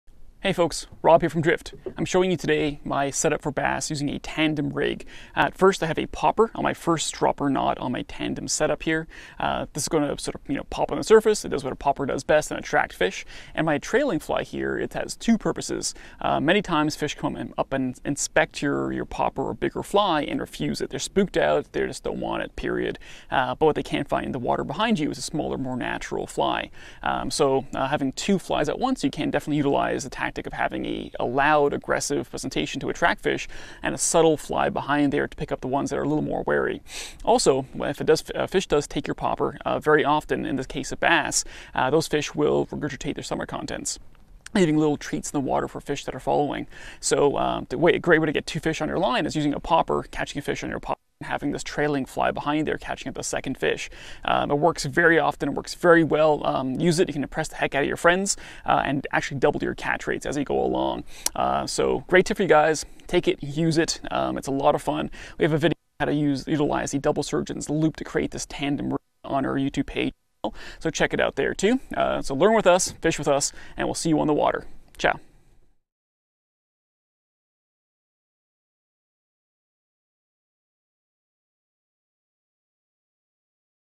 Tandem (Dropper) Rigs For Bass - How & Why We Use Them - Drift Outfitters & Fly Shop
Two flies for bass!  Yes, you read that right!
This technique is not often utilized, but it is responsible for countless double headers on a singe rod (that's two fish at once!)!

Take advantage of their curiosity and their willingness for a free and easy meal, watch the video and find out what we mean!

Learn and Shop With Us! Shipping Coast to Coast to Coast